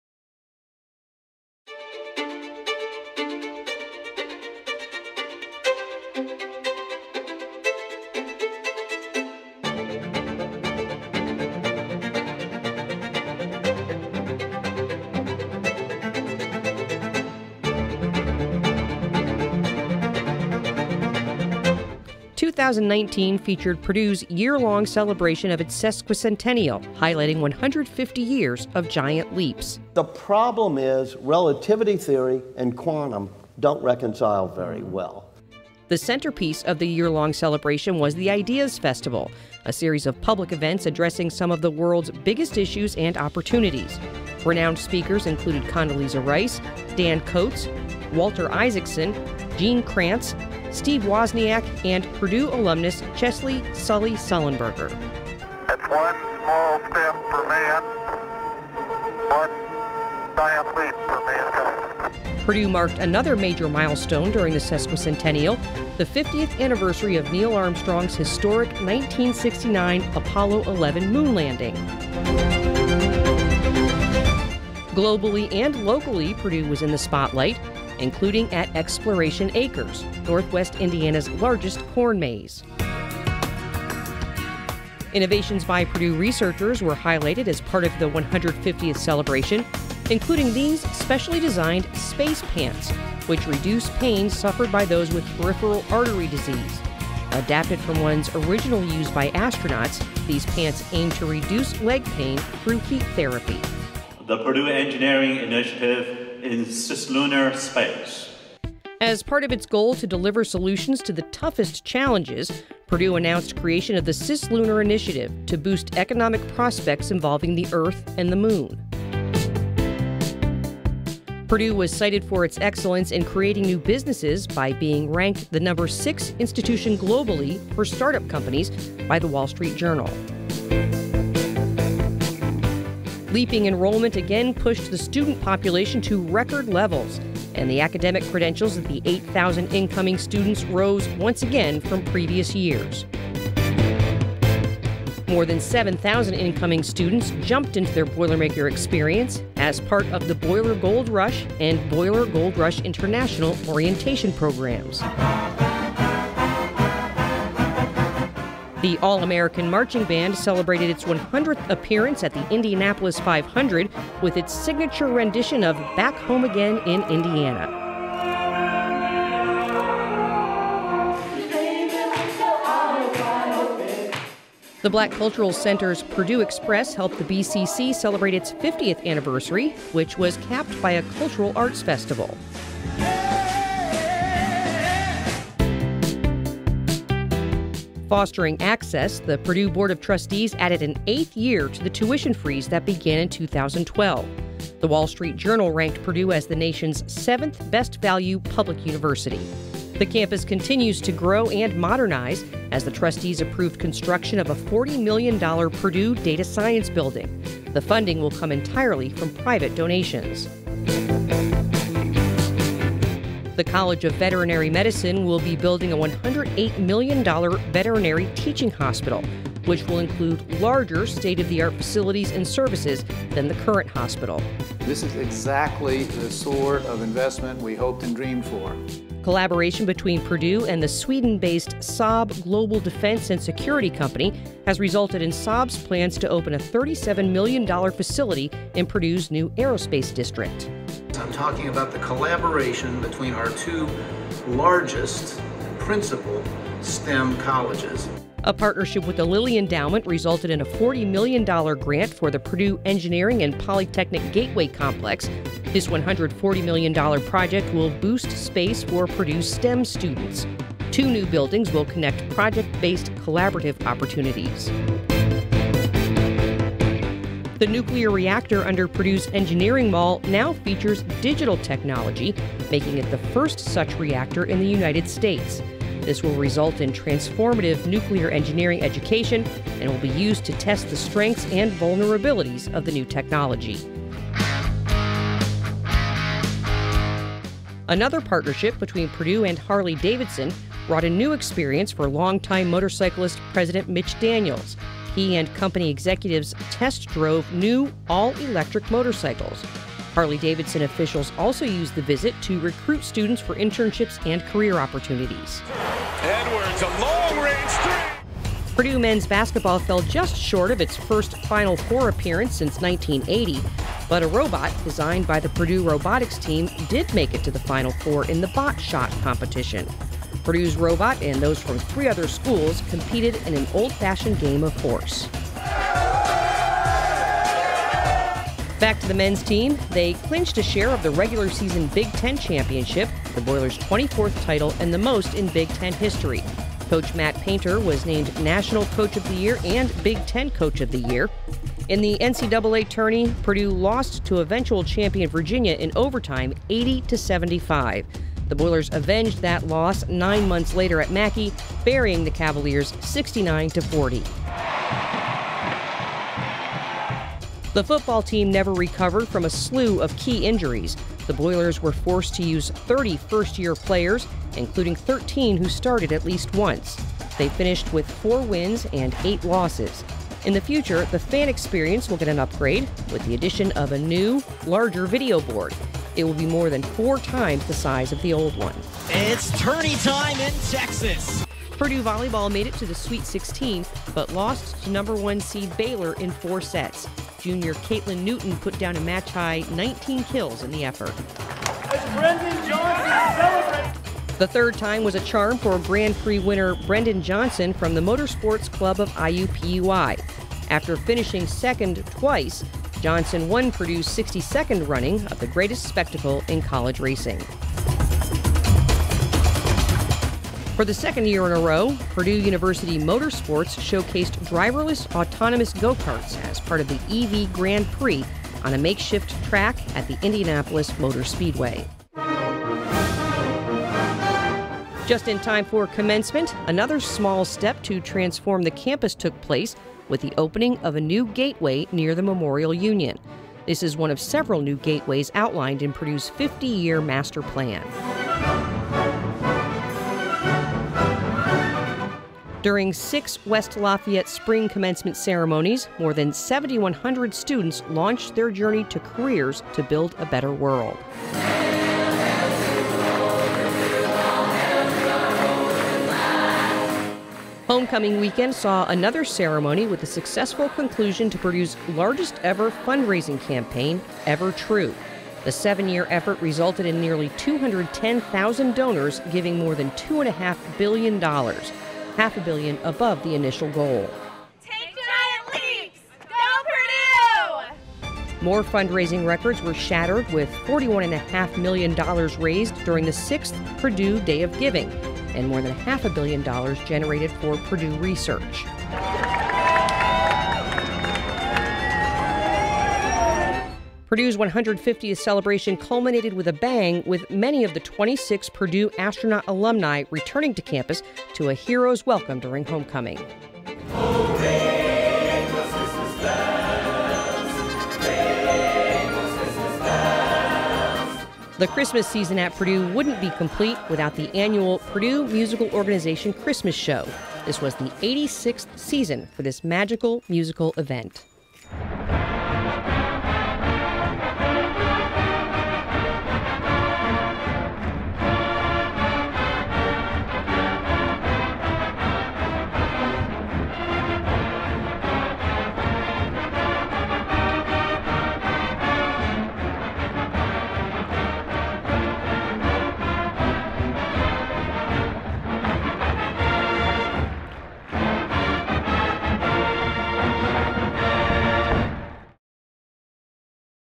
2019 Purdue Year in Review
This recording is part of the Purdue University Newsreels and Year in Review recordings. Identifier Number: UA65av0065.